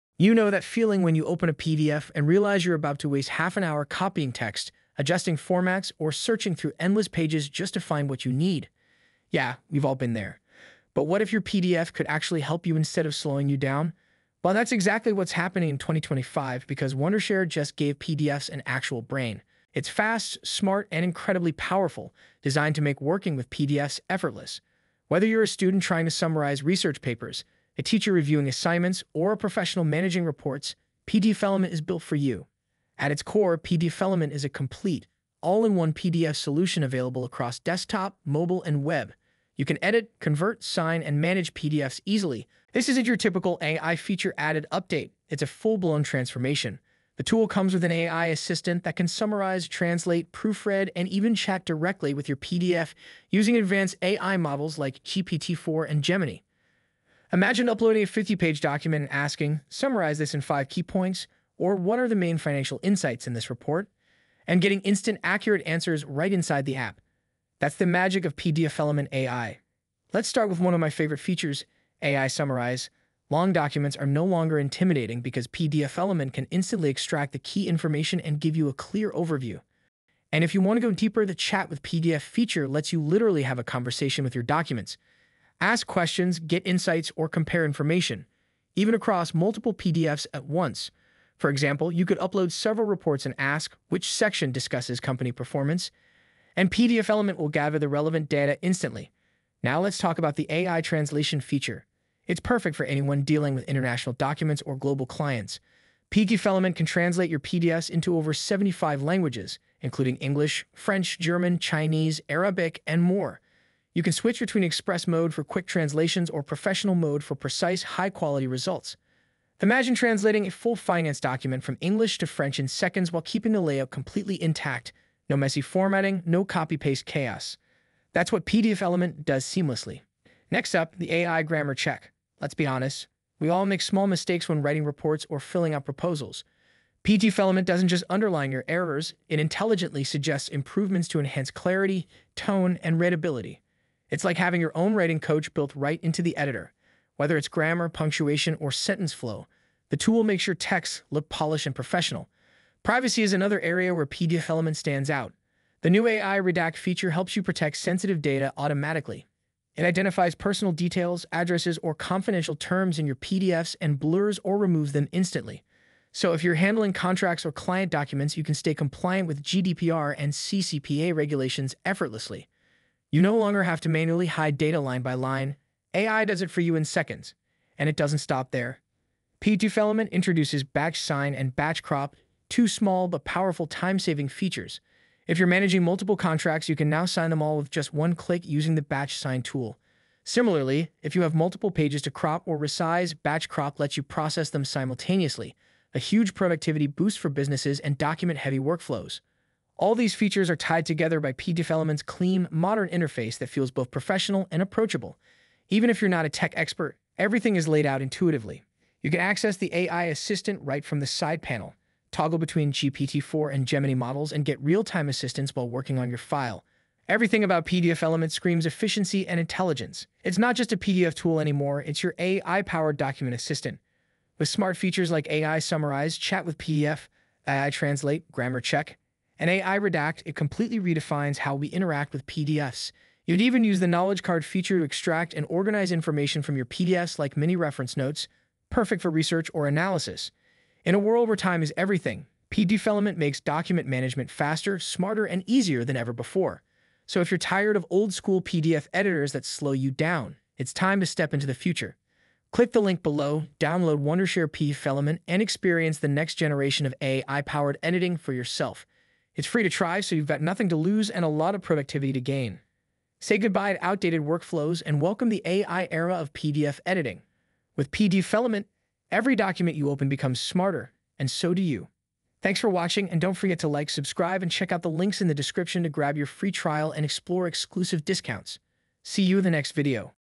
Say GOODBYE to Dull PDFs with Best AI tool — PDFelement
0:00 Intro
0:57 AI Summarize
1:40 AI Translation
2:11 AI Grammar Check
2:33 Smart Redact
3:00 Batch Sign/Crop
3:29 AI Assistant
3:53 AI Knowledge Card
4:07 Final

Unlock the full potential of your PDF files with PDFelement, the ultimate PDF editor. Say goodbye to boring PDFs and hello to a world of creative possibilities. With PDFelement, you can easily edit, convert, annotate, batch sign, redact, and batch crop your PDF files, making them more engaging and interactive. Discover how to add AI summary, mindmap, and signatures to your PDFs, and learn how to use PDFelement's powerful tools to enhance your documents. Learn more about PDFelement's features and benefits, and start creating stunning PDFs today.